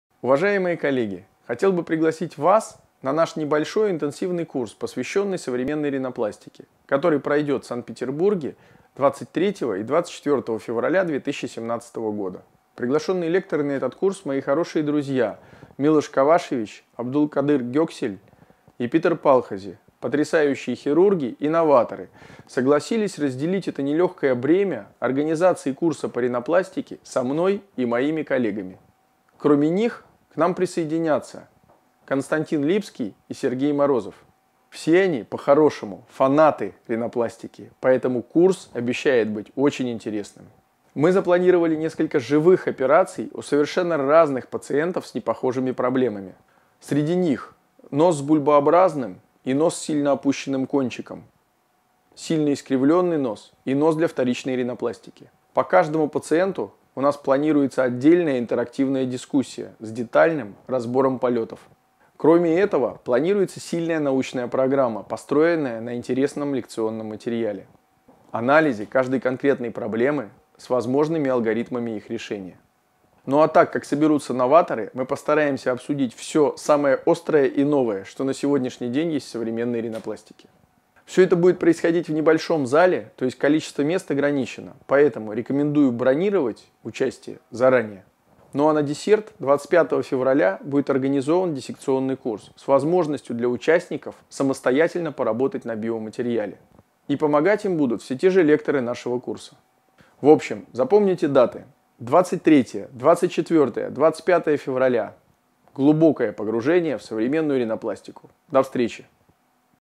Виталий Жолтиков: приглашение на интенсивный курс
Интенсивный курс: «Современная ринопластика: эффективные методики и инновации». 23–24 февраля 2017 г., Санкт-Петербург.